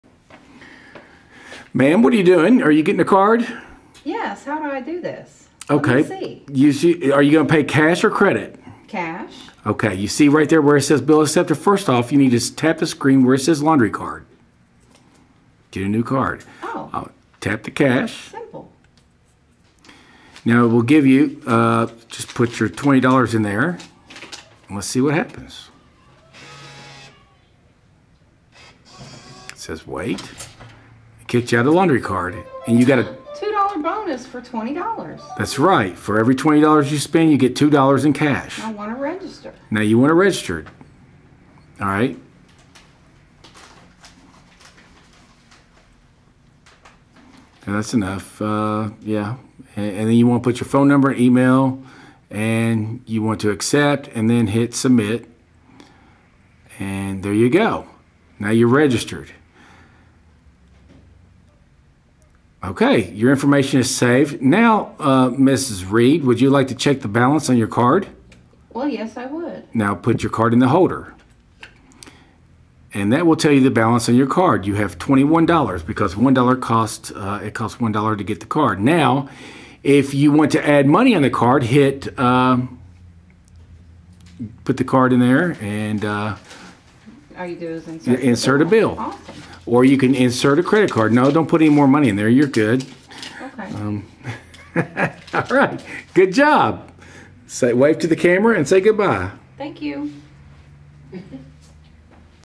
Laundroworks all card system in a Laundromat ease of use
Here is the system that Splas Em Out is changing over from quarter to card only.  It's time, and our customers are very happy that we have made the switch to card.  Here is a little sneak peak on how easy it is to use.